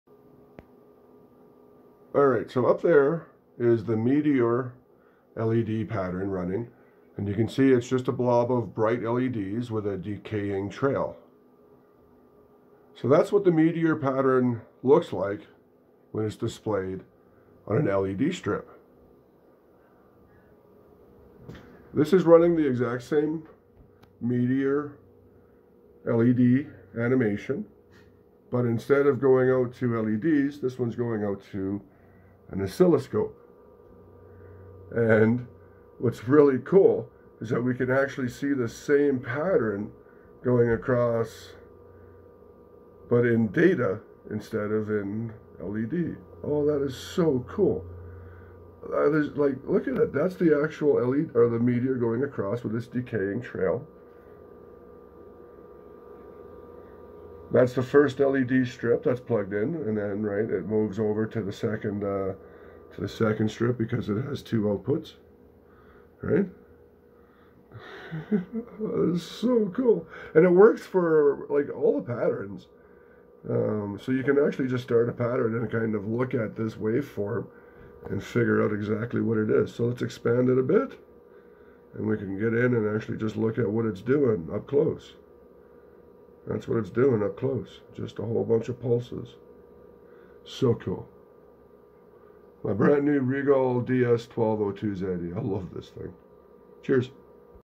Display LED strip data on a oscilloscope.
This shows a neat phenomena where the waveform of a data on an oscilloscope matches what that data looks looks like on a LED strip itself.  It's a very neat effect.